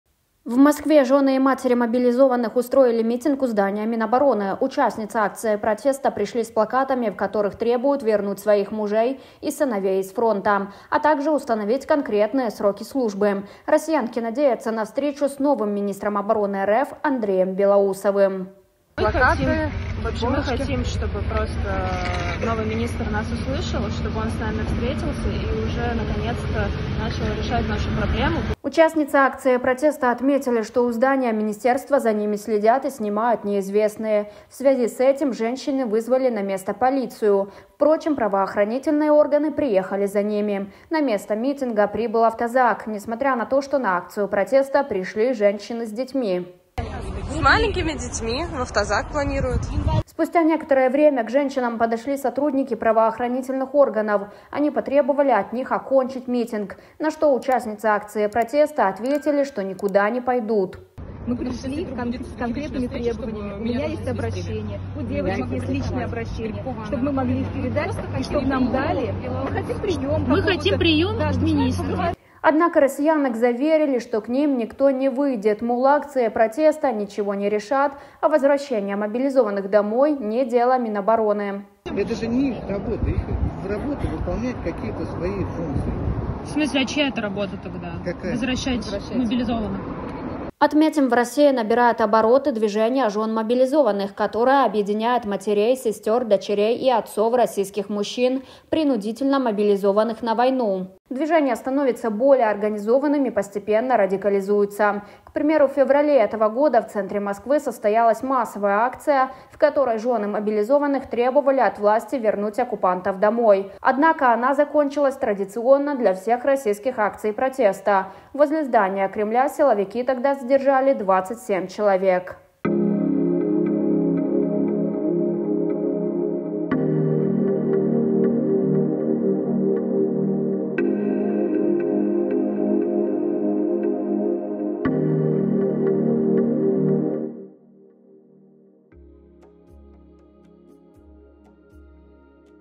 ⚡️Прямо сейчас! Протесты в центре Москвы. Разъяренные россиянки пришли к Минобороны
RU - В Москве прошла акция протеста под зданием Минобороны РФ. Туда съехались матери и жены мобилизованных, в ответ к ним пригнали силовиков и автозаки.

UA - У Москві відбулася акція протесту під будівлею Міноборони РФ. Туди з'їхалися матері та дружини мобілізованих, у відповідь до них пригнали силовиків та автозаки.